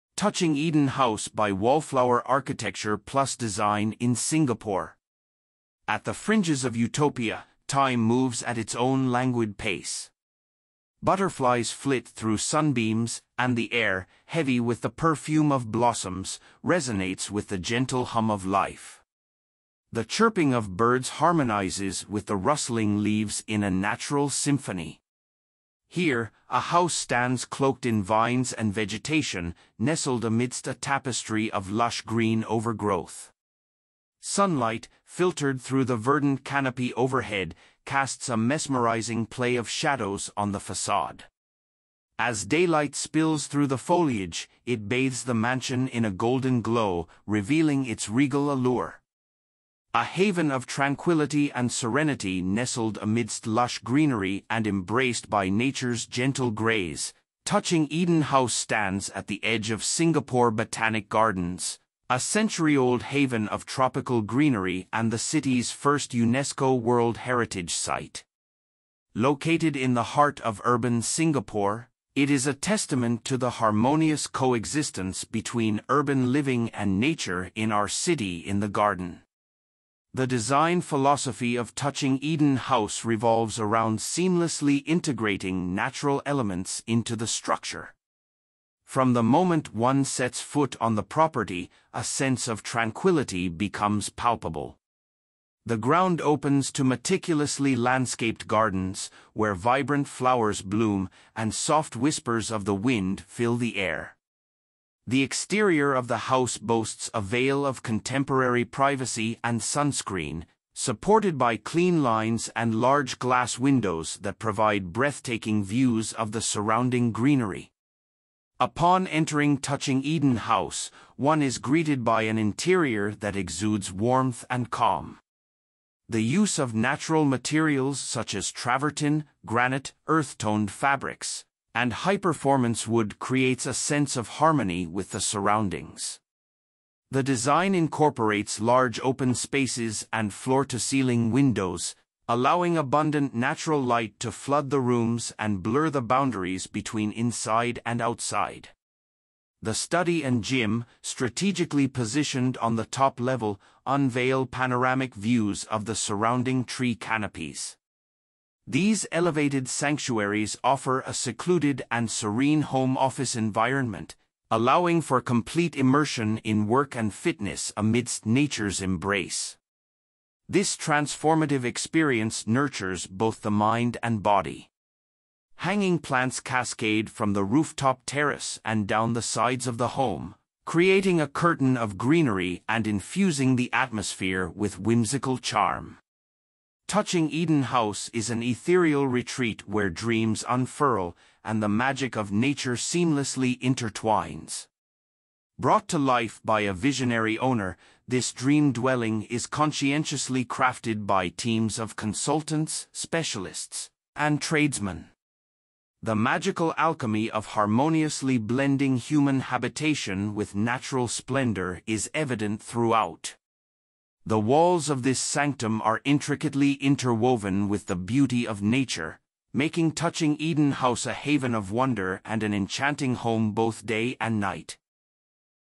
Touching Eden House By Wallflower Architecture + Design In SINGAPORE
At the fringes of utopia, time moves at its own languid pace. Butterflies flit through sunbeams, and the air, heavy with the perfume of blossoms, resonates with the gentle hum of life. The chirping of birds harmonizes with the rustling leaves in a natural symphony. Here, a house stands cloaked in vines and vegetation, nestled amidst a tapestry of lush green overgrowth. Sunlight, filtered through the verdant canopy overhead, casts a mesmerizing play of shadows on the façade. As daylight spills through the foliage, it bathes the mansion in a golden glow, revealing its regal allure.

Archi Studio

Architects: Wallflower Architecture + Design
Area:
1789 m²
Year:
2023
Photographs:Finbarr Fallon
Manufacturers:  Gaggenau, Gessi, OTIIMA, Berker, Big Ass Fan, Giorgetti, Kone, Moooi
Engineering: MSE Consultants Pte Ltd
MEP Engineering: ALLZ Consulting Services
Landscape Architects: Salad Dressing
Lighting Designers: Light Collab LLP
Interior Designers: Edmund Ng Interior Studio
Greenery: Nyee Phoe Flower Garden Pte Ltd
Contruction: Good Class Builders Pte Ltd.